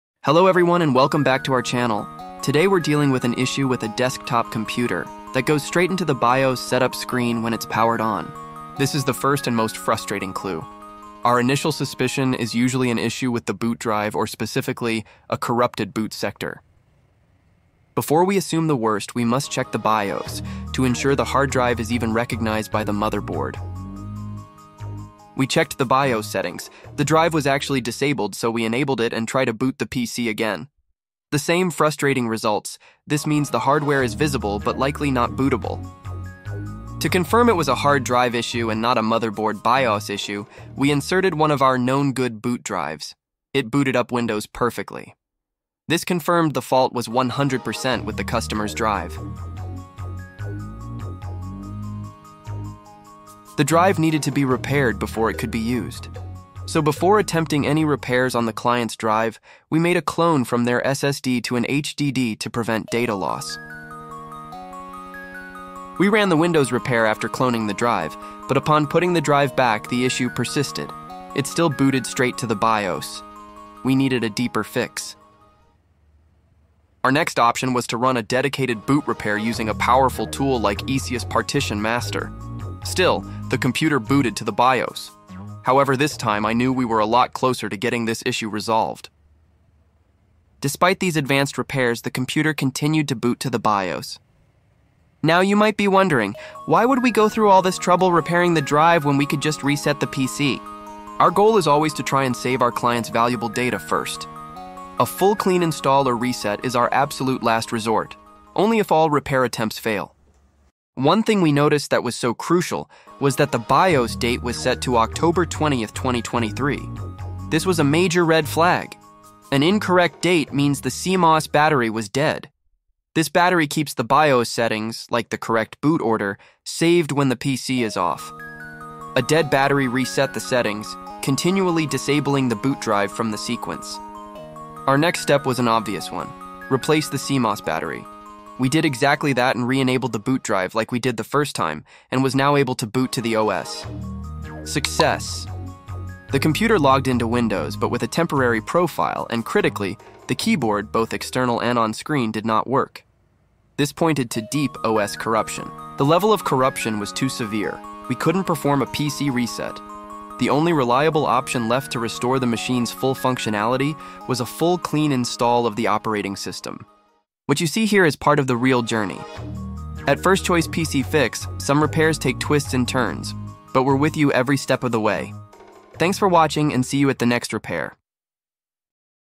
Fixing a Desktop that Boots Straight to BIOS
Today, we have a desktop computer on the bench with a classic, frustrating symptom: every time it’s turned on, it skips Windows and boots straight into the BIOS setup screen.

What seemed like a straightforward failed hard drive turned into a multi-layered diagnostic puzzle. In this video, I walk you through the entire process—from initial suspicion to the final clean install—and show you why you should never assume it's just one thing wrong with an older PC.

We tackle data preservation first, deal with failed repair tool attempts, and discover a hidden culprit (a dead CMOS battery resetting boot orders) that was complicating the whole repair.

Highlights of this repair:

Diagnosing the "Straight to BIOS" boot loop.

Why we prioritize cloning the drive BEFORE attempting fixes.

The failure of standard boot repair tools (Windows & EaseUS).

Finding the hidden issue: How a $2 CMOS battery caused total chaos.

Dealing with severe OS corruption and temporary profiles.

Timestamps: 0:00 The Symptom: BIOS Boot Loop 0:15 Initial Diagnosis & Isolating the Fault 0:35 The Priority: Data Preservation First! 0:45 Failed Drive Repair Attempts 1:15 The Hidden Culprit Discovered (CMOS) 1:45 The Final Hurdle: OS Corruption & Clean Install